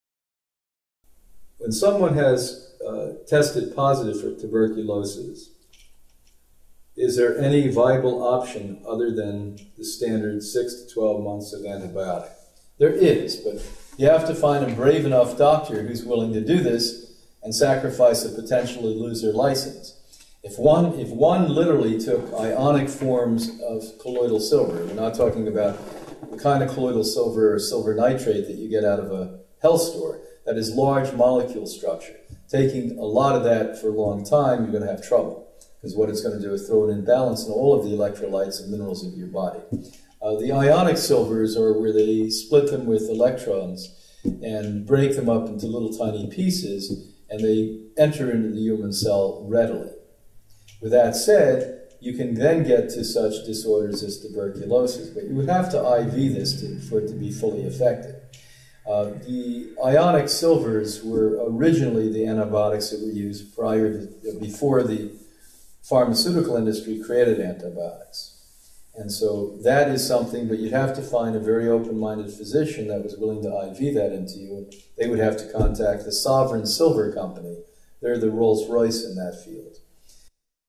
Treating Tuberculosis
Dr. Clement Discusses Treatment of Tuberculosis